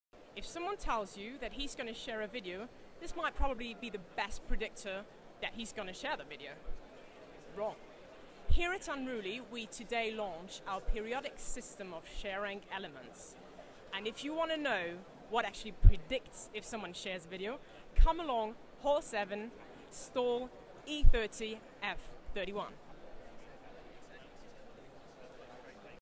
DMEXCO14 - Does the intention to share predict sharing?
Do people as they say?
This is part of our brand new Periodic Table of Unruly ShareRank elements.